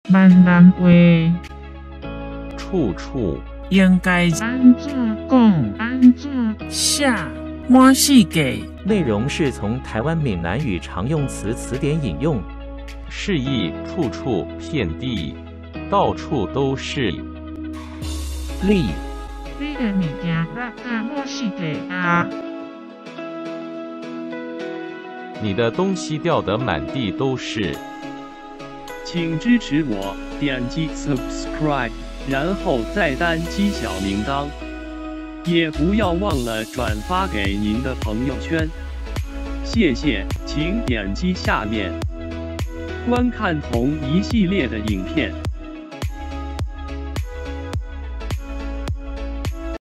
閩南語 ~ 處處 ~ 應該怎樣講， 怎樣寫？
(第441集)
類似答案很多，請先給您 的答案，再看影片答案是否跟您的一樣。提供的僅是其中比較特殊之一。
各位鄉親，咱兮母語是閩南語，教育咱兮兒女序細講母語是咱兮責任。